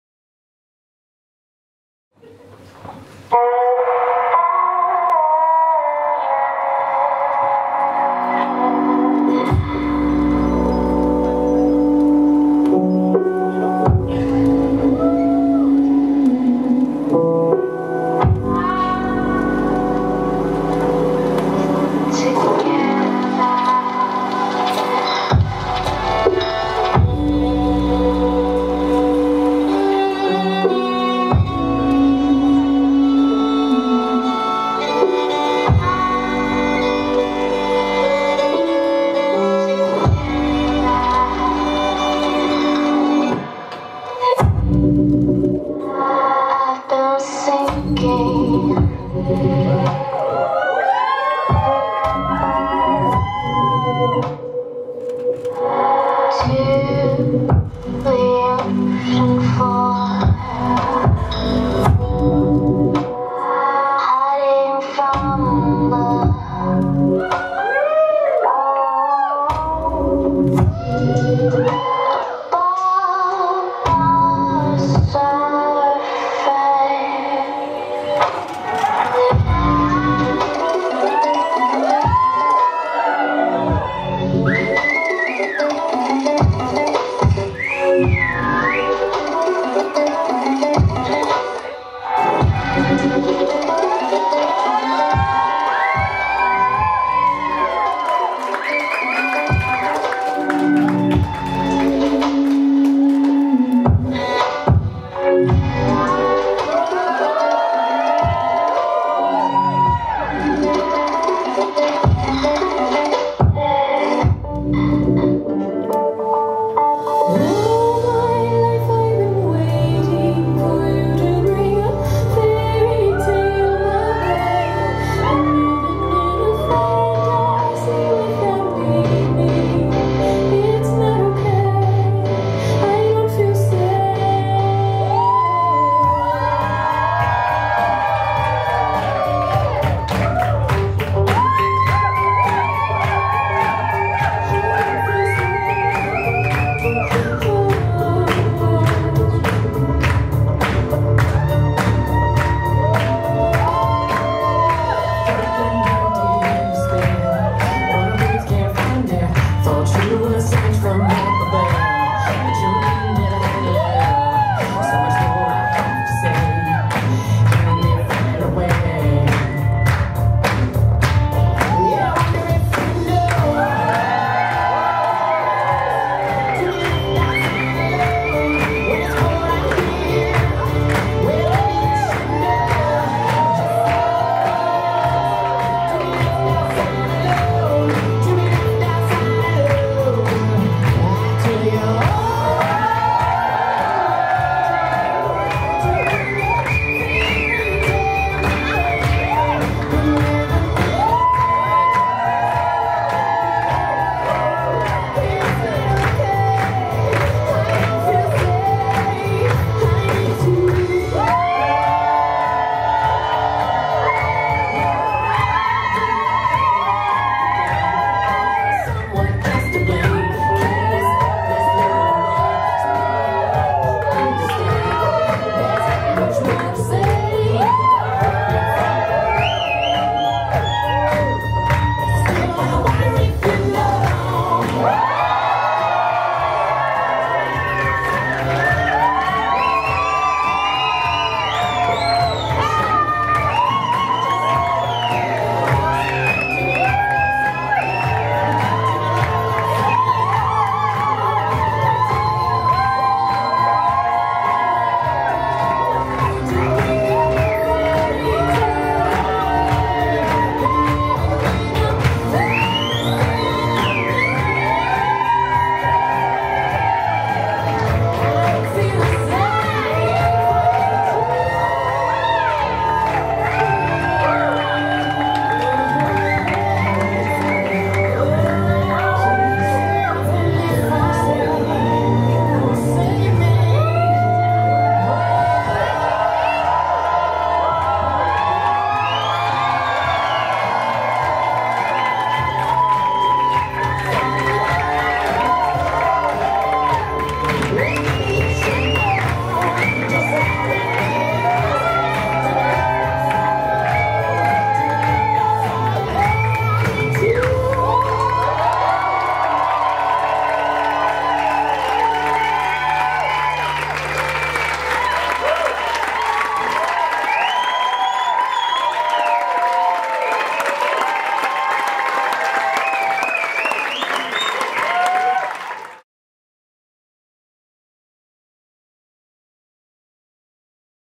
Mystic Angel " Birdcage" - Burlesque Show
Swiss Burlesque Academy Show at Citrons Masqués in Switzerland - January 10th, 2020

Creation: Mystic Angel

Coach: Misty Lotus